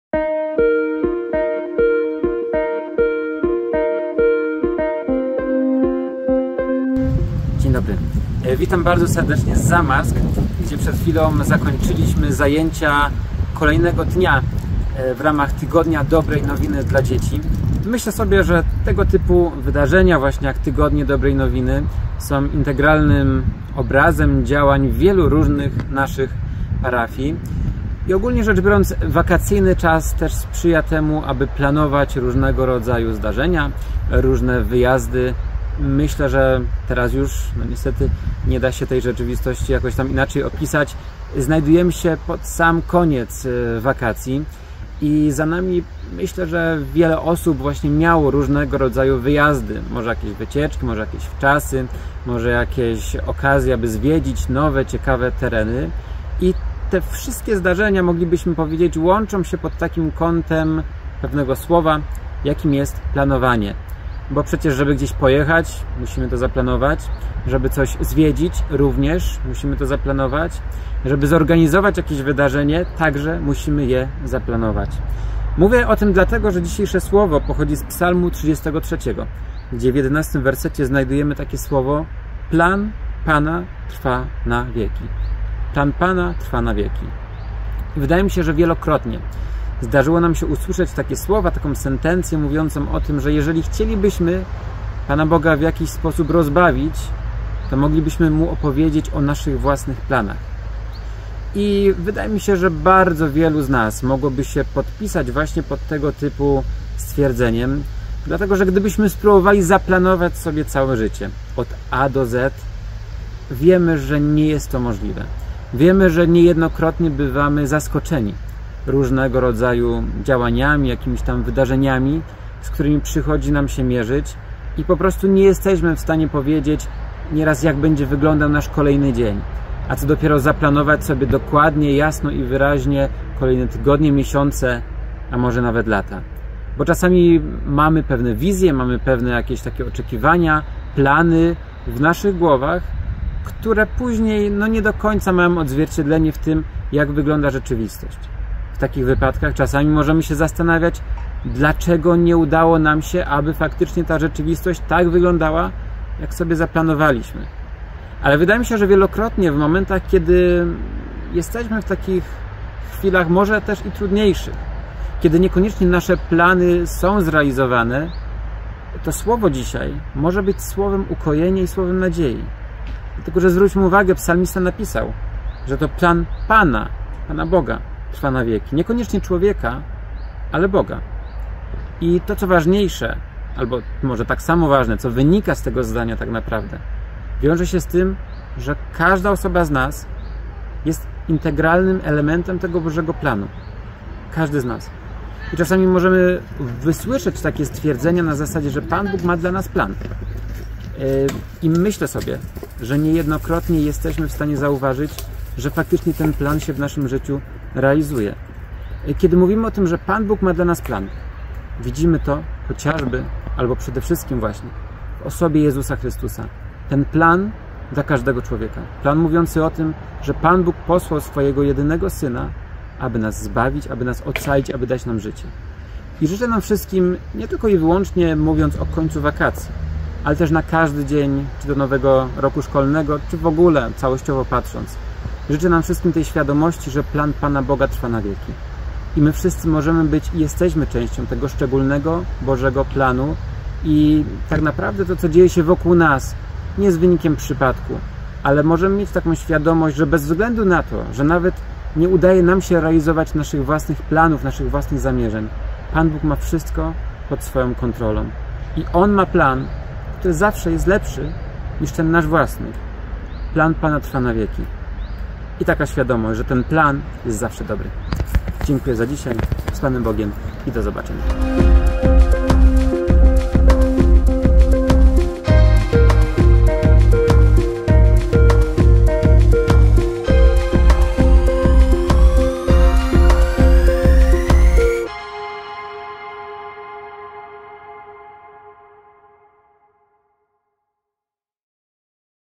Codziennik biblijny, słowo na dzień 30 sierpnia 2024 r.
ks. Mateusz Mendroch, Parafia Ewangelicko-Augsburska w Cieszynie
Rozważanie biblijne na Słowo dnia:

Plan Pana trwa na wieki.
Księga Psalmów 33,11
Codziennie o 7 rano spotkanie z Biblią.